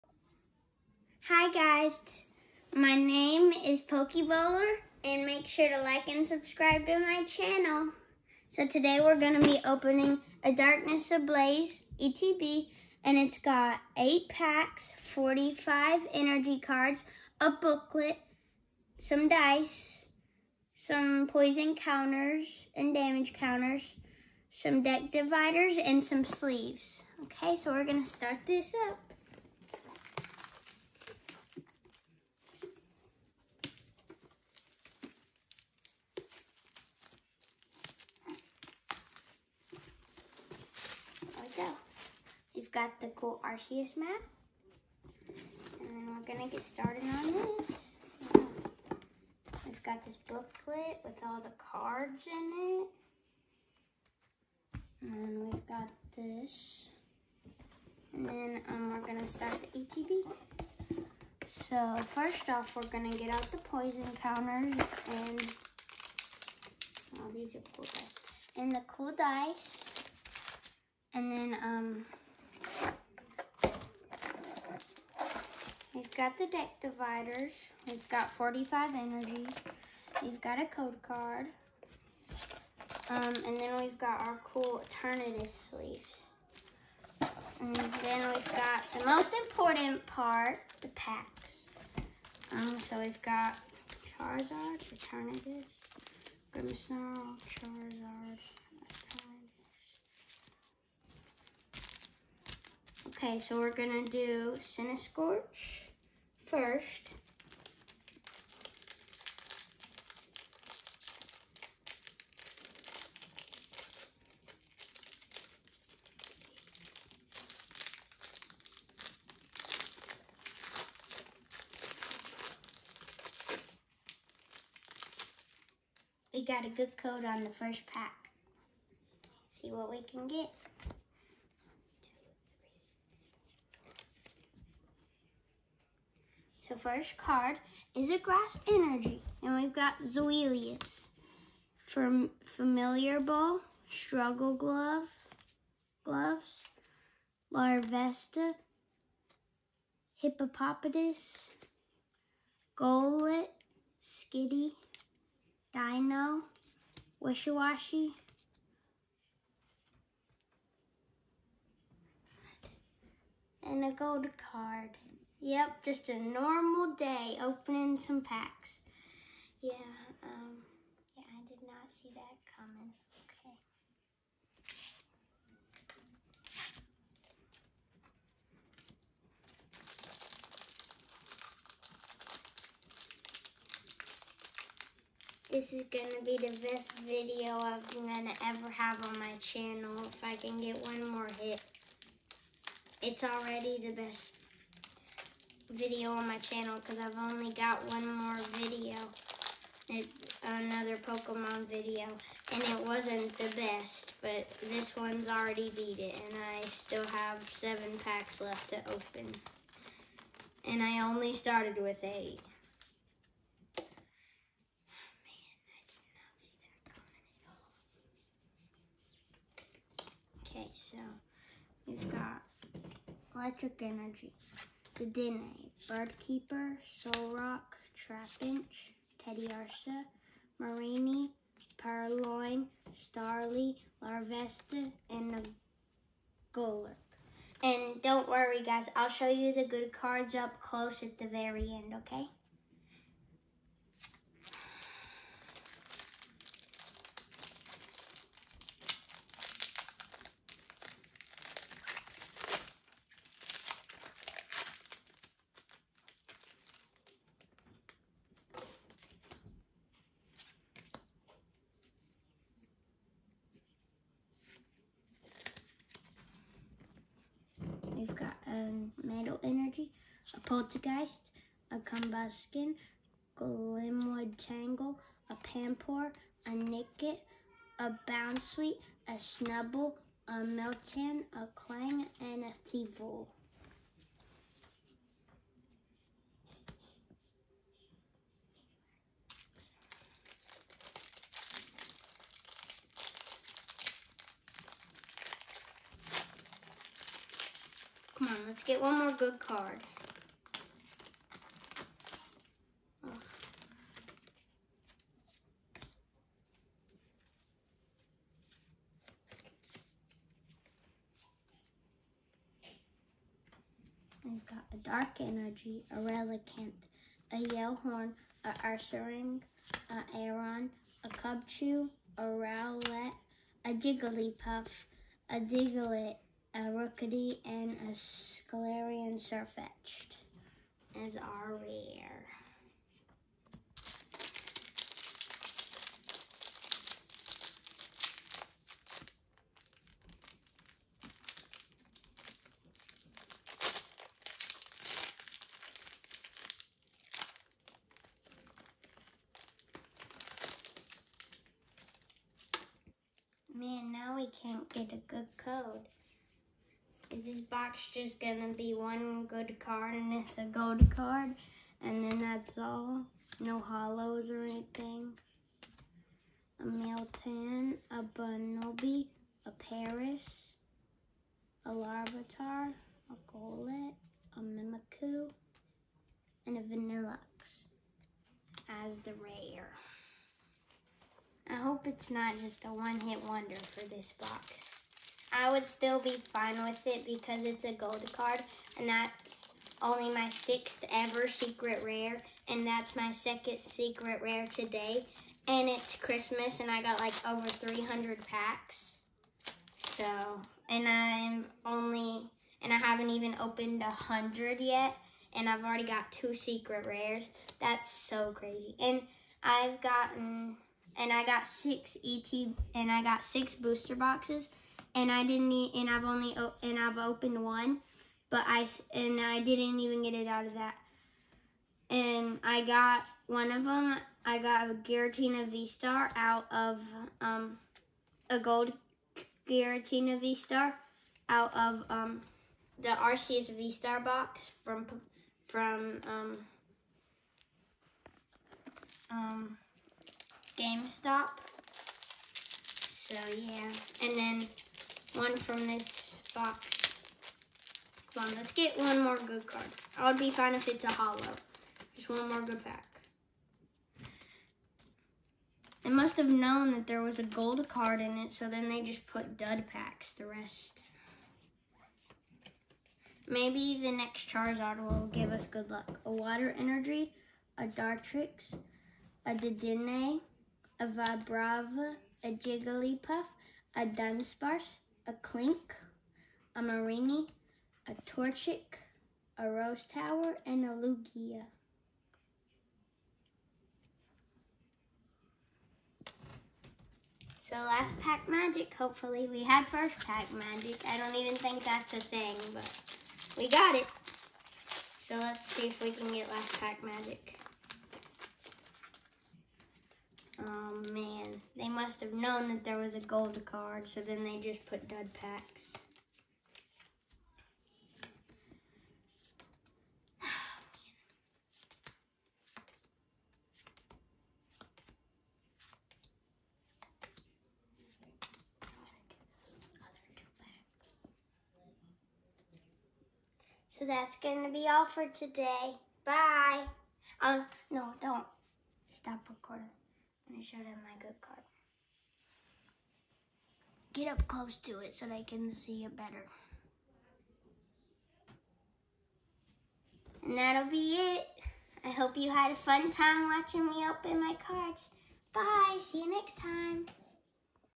Darkness Ablaze ETB!
Opening a Darkness Ablaze Elite Trainer Box!

Come see what’s inside! 👀👀